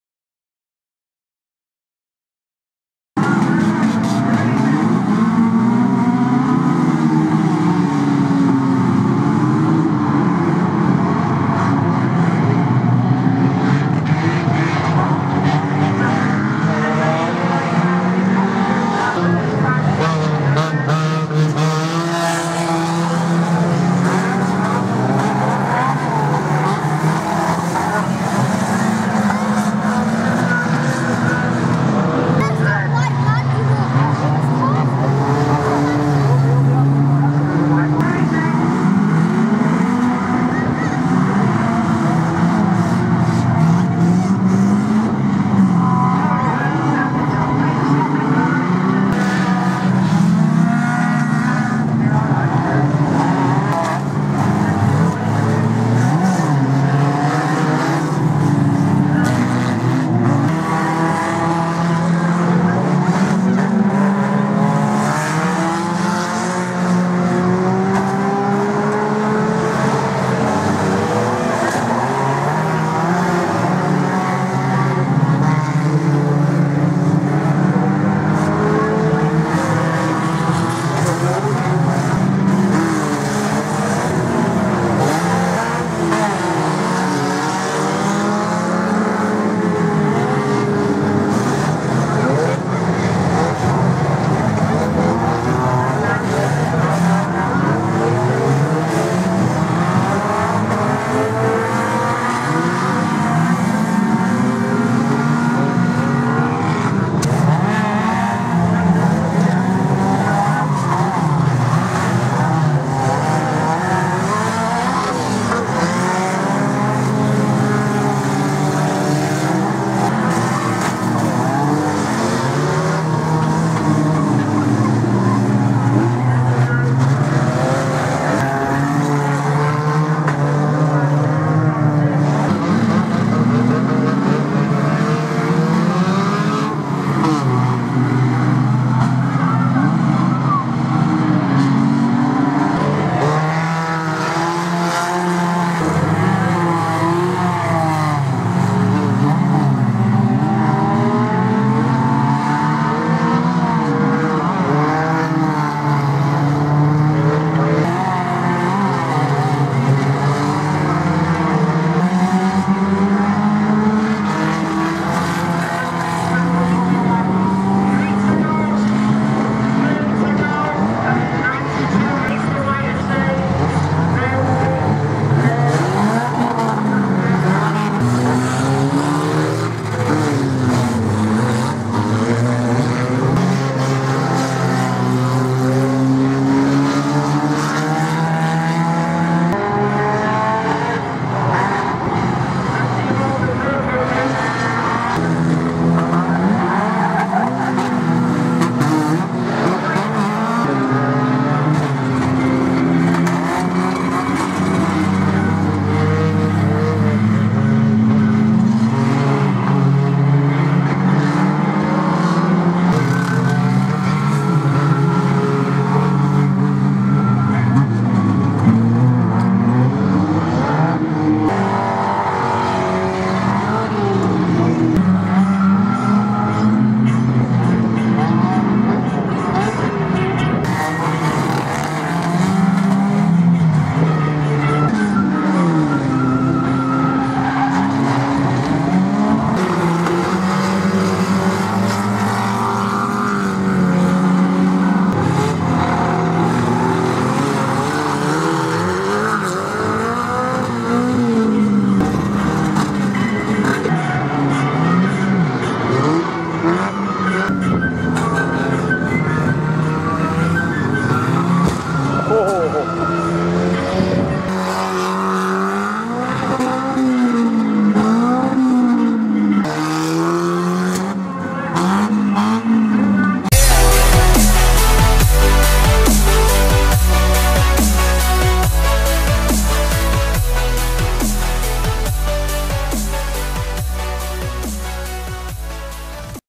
Buxton Raceway Bomber Teams 2014
This is the last event at Buxton Raceway for 2014, the annual Bomber Teams. This year they went back to the Red Vs White format that worked brilliantly. It's seemed that every one had a great time with hand shakes all round.
Camera: Panasonic HC-V110
Editing Software: Cyberlink Power Director 12